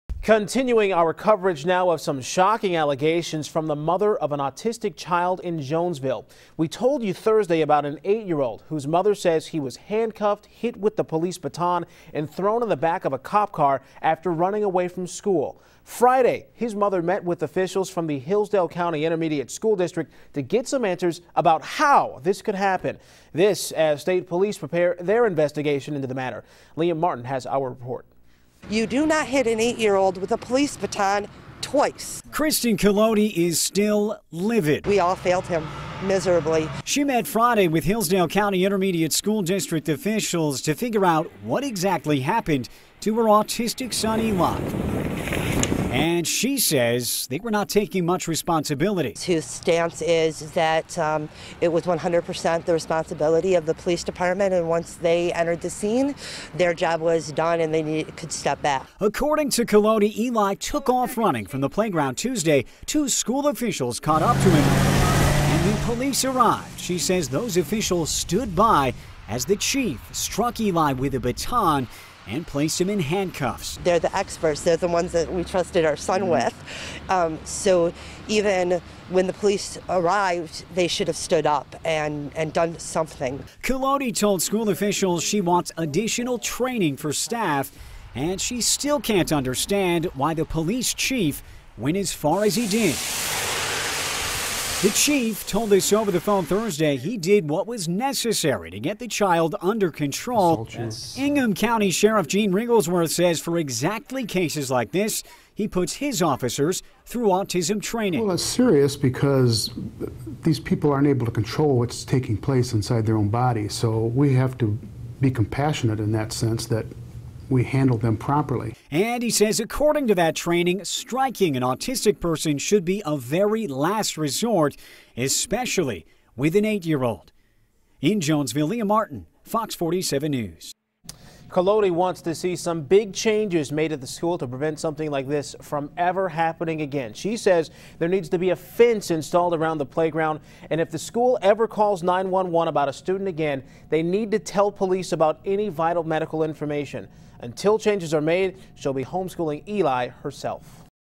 Mother Wants Answers After Autistic Son Is Struck by Police
School officials and Michigan State Police say they're conducting full reviews.